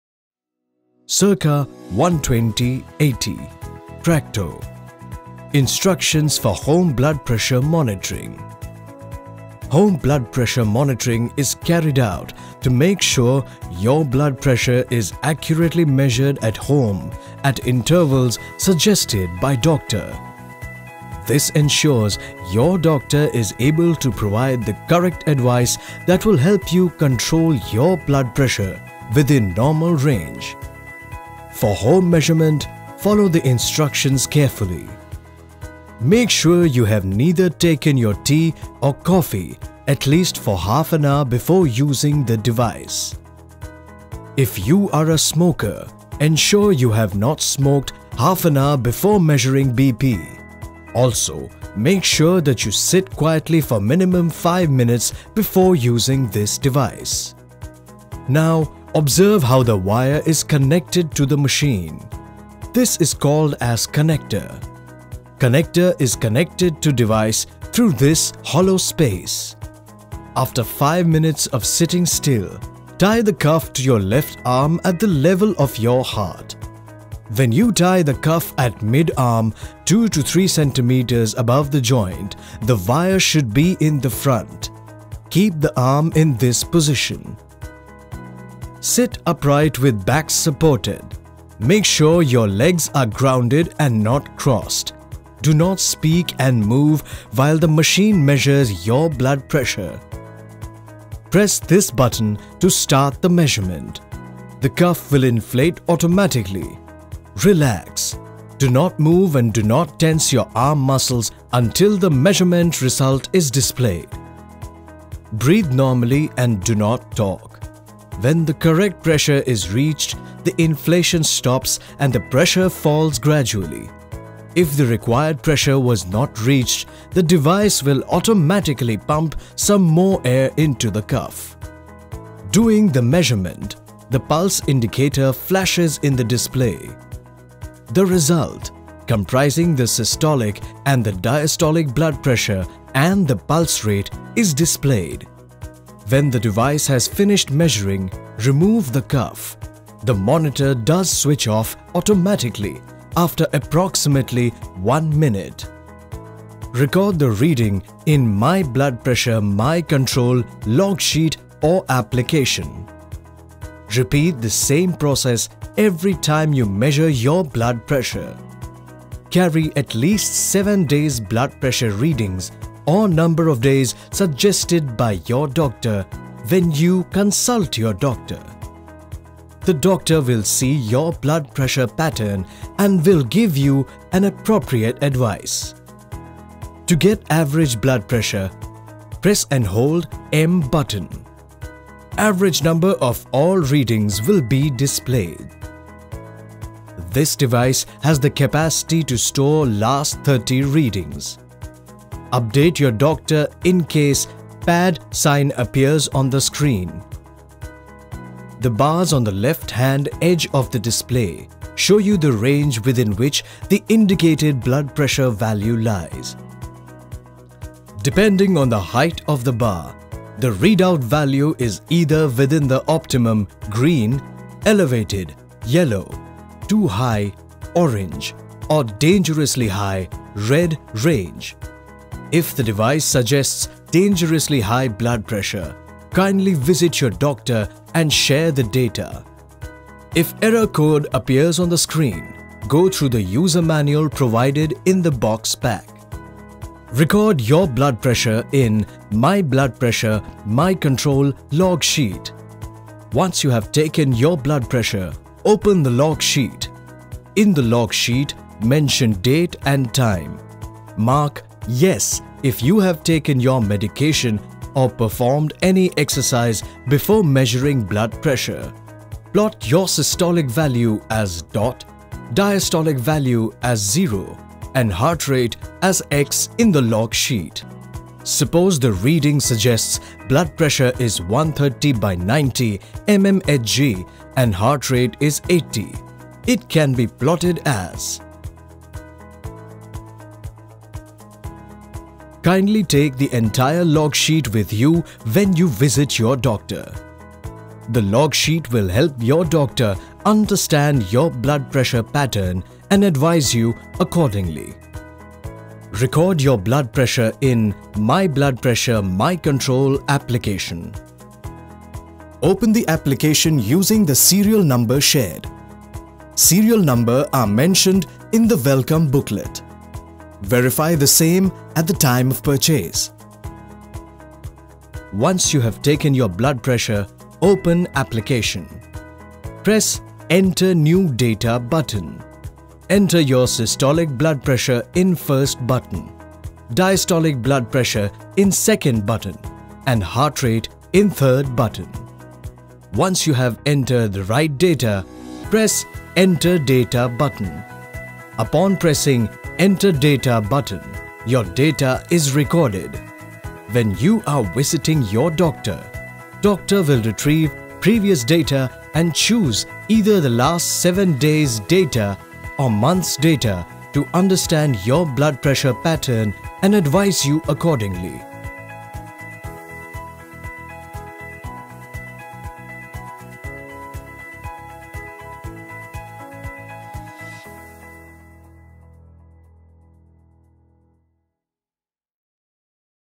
How to measure blood pressure with Home BP monitors
There are a few precautionary measures one need to take before measuring the blood pressure. Here is a simple illustrative of how to get an accurate blood pressure reading at home with a validated device and precautions before measuring the pressure.